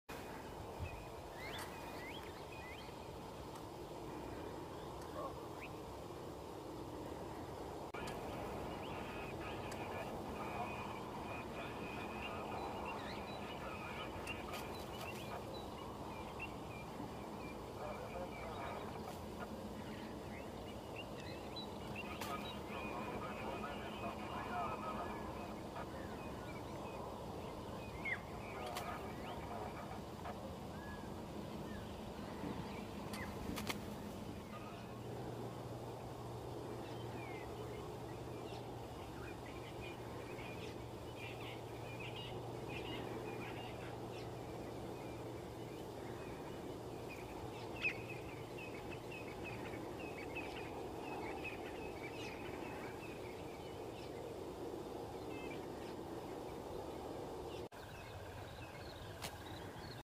Burchell's Coucal attacks a chameleon
Burchell's Coucal tries everything to get this massive prize off the branch but the chameleon holds on relentlessly...